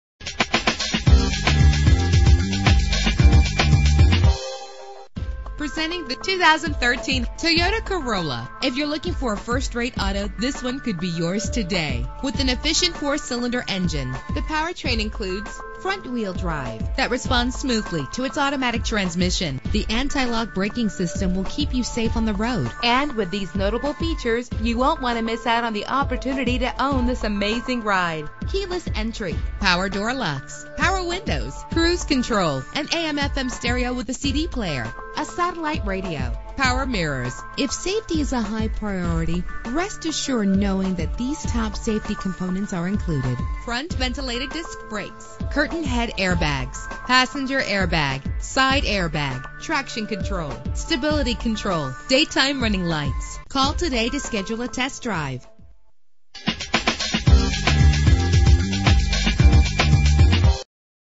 2013 Toyota Corolla - Leesburg FL
Year: 2013
Make: Toyota
Model: Corolla
Trim: LE
Engine: 1.8 liter inline 4 cylinder 16 valve
Transmission: 4-Speed Automatic
Color: Magnetic Gray Metallic
Mileage: 7
Address:
8629 US Hwy 441